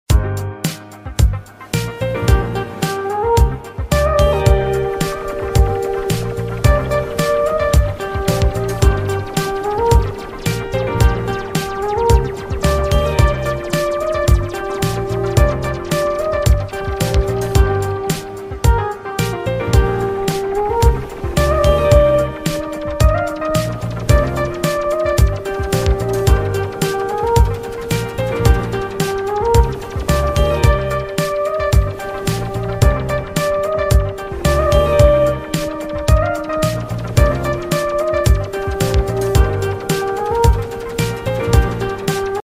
GiỚI THIỆU TPSHELL - BEACH ART | INTRODUCTION
Chúng tôi - TP Shell đước thành lập lần đầu vào năm 1998, mang sứ mệnh đưa vẻ đẹp của biển cả, đại dương về ngôi nhà của bạn, từ các sản phẩm mỹ nghệ làm đẹp cho bản thân đến các sản phẩm decor nội thất, làm đẹp cho ngôi nhà, không gian của bạn, TP Shell sẽ giúp bạn như đắm mình vào biển cả và mang cả âm hưởng đại dương từ trong chiếc vỏ ốc.
 🌊TP Shell - Bring the sea to your life🌷
🏡TT5 TOCEPO
🏡18D Nguyễn Huệ, Quy Nhơn, Bình Định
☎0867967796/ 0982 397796
© Bản quyền thuộc về TP Shell - Beach Art
© Copyright by TP Shell - Beach Art ☞ Do not Reup